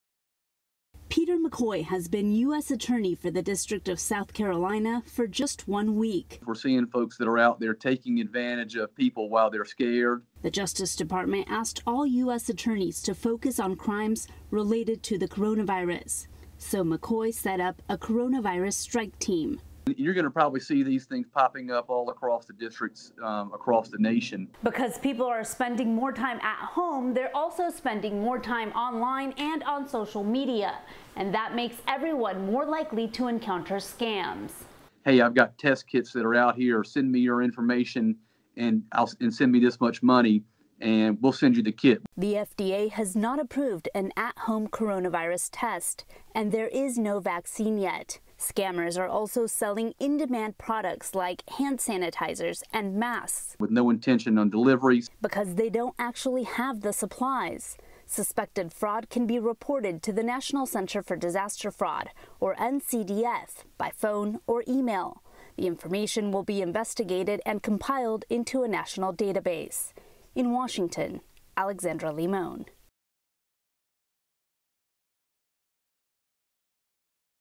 Attorney's offices nationwide tasked with investigating coronavirus scams
The Justice Department asked U.S. attorney’s offices across the country to focus on investigating fraud and scams related to the coronavirus pandemic.